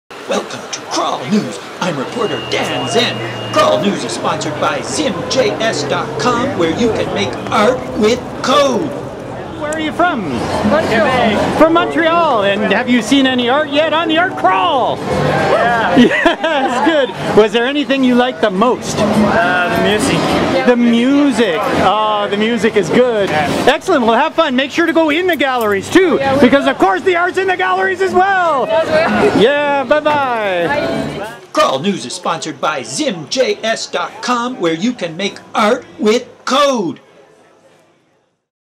Crawl News 13 (Visitors from Quebec) - Sponsored by ZIMjs.com - make art with code!
where you can make art with code!  Crawl News is a mini-series featuring Hamilton Art Crawl interviews - more to come!